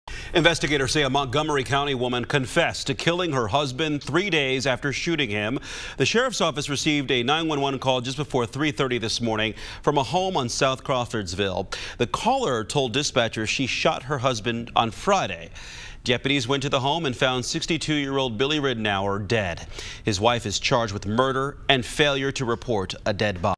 Woman shot, killed husband and waited the weekend before calling police
A woman is accused of shooting and killing her husband and then waiting the weekend before calling the police to report it.